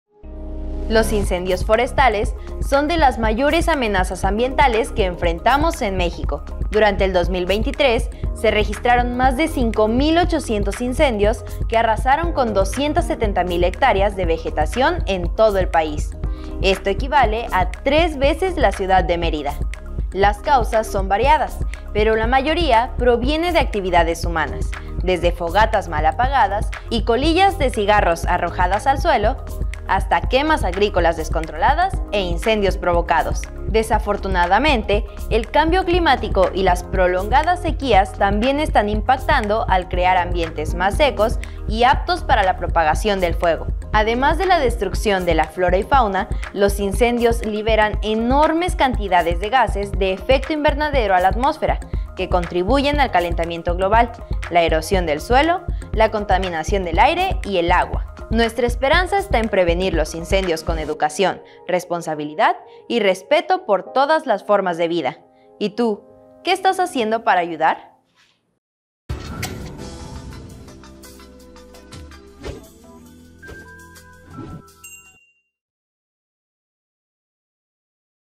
Breves: "Incendios forestales"
¿Sabías que los seres humanos somos responsables de 4 de cada 5 incendios forestales en el mundo?

Los incendios forestales son uno de las mayores amenazas ambientales que enfrentamos en México. Durante el 2023 se registraron más de 5,800 incendios que arrasaron con 270,000 hectáreas de vegetación en todo el país, esto equivalente a 3 veces la ciudad de Mérida.

*Episodio 400

Una producción de Vientos Culturales A.C.
Investigación y guion: Ulises Soto Ruiz
Diseño y animación: José Roberto Flores Roa